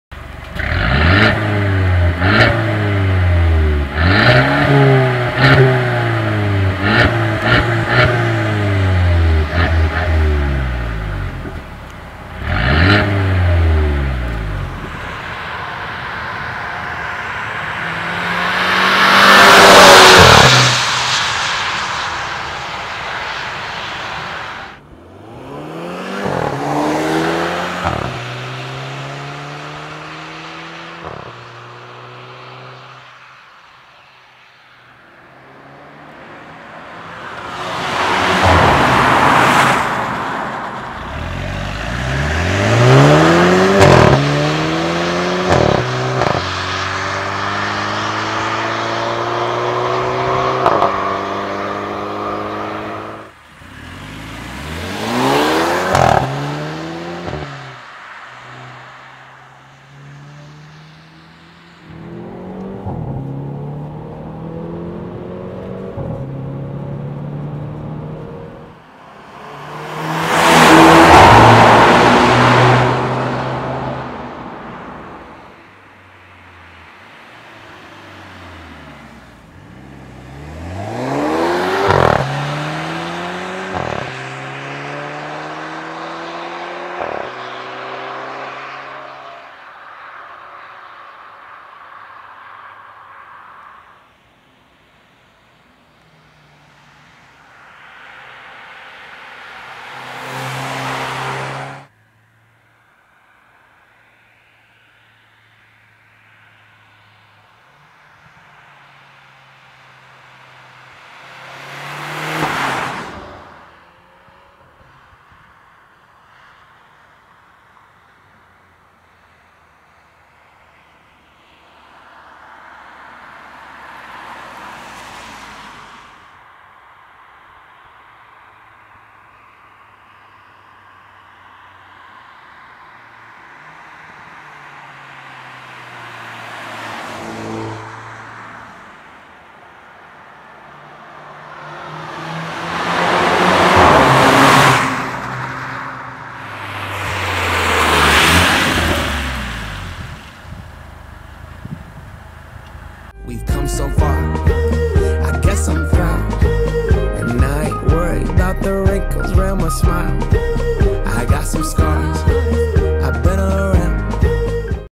2017 Mercedes-Benz CLA 250 Sport Shooting Brake - Pure sound, Drive and Sound, Testdrive
2017 Mercedes-Benz CLA 250 Sport Shooting Brake

Hier schon einmal vorab unserem Video zum CLA ein pures Soundvideo... Viel Spaß:)